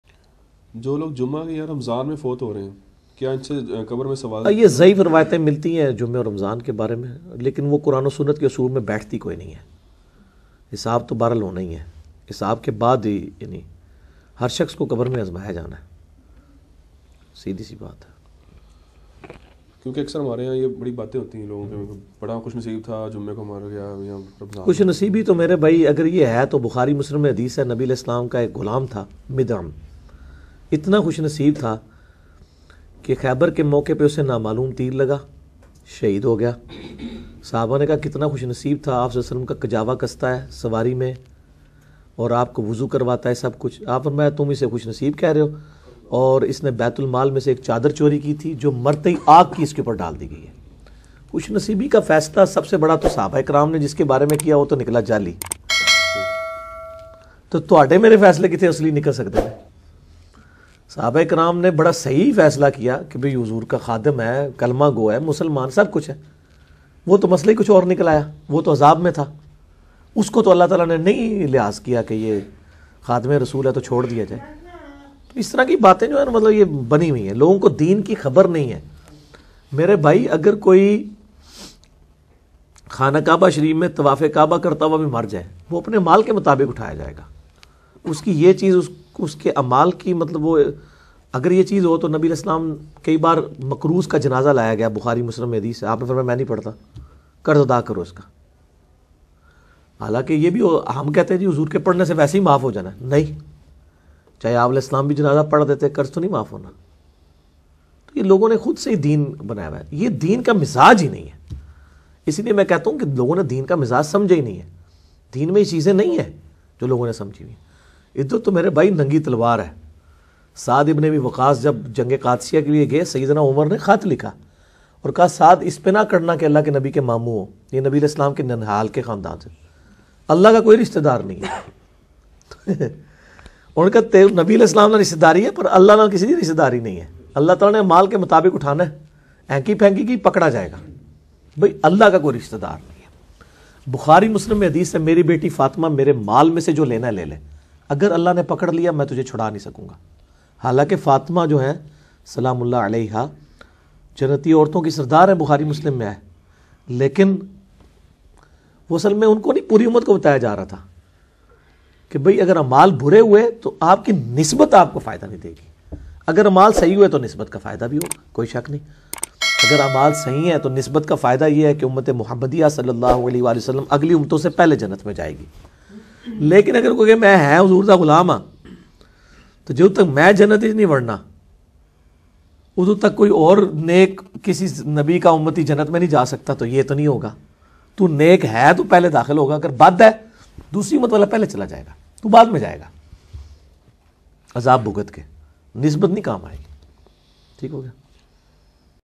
Ramadan Main/Jumma K Din Marny Wala Jannati? | Engineer Muhammad Ali Mirza Q&A Session
Video Cradits: @Engineer Muhammad Ali Mirza - Official Channel
This video is about detailed discussion on if a person dies in Ramadan or on Friday, will he directly go to Jannah without any questioning?
Jumma k din marny wala seedha jannat main jye ga?
Ramadan main marny wala seedha jannat main jye ga??